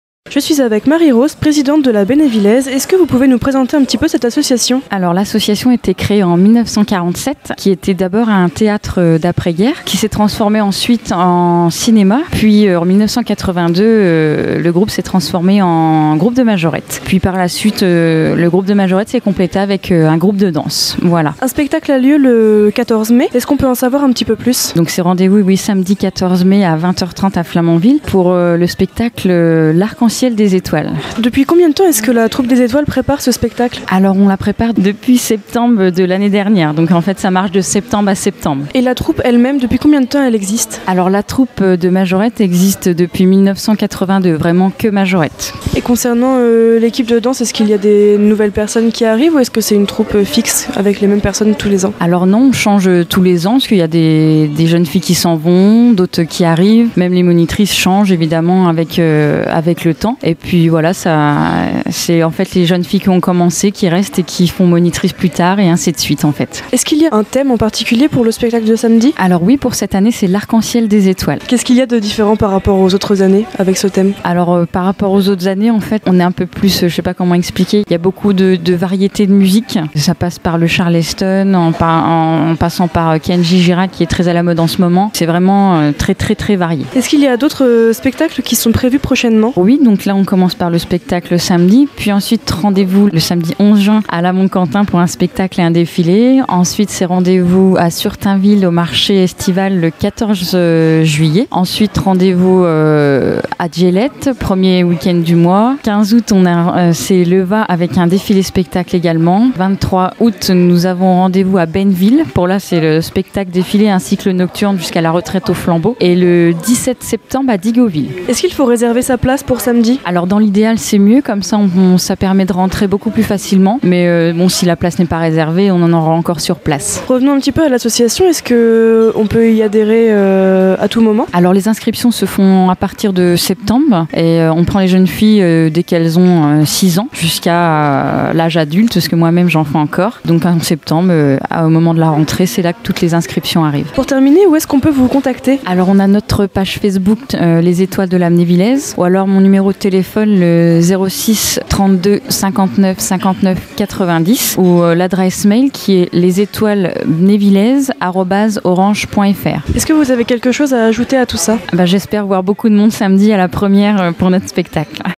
Radio HAG' FM - Les étoiles de la B'Névillaise
La troupe des étoiles de la B'Névillaise, composée de majorettes et de danseuses, vous convie à son spectacle d'ouverture le samedi 14 mai à Flamanville. Marie-Rose Laniepce, présidente de l'association, au micro de Clara, pour HAG' FM.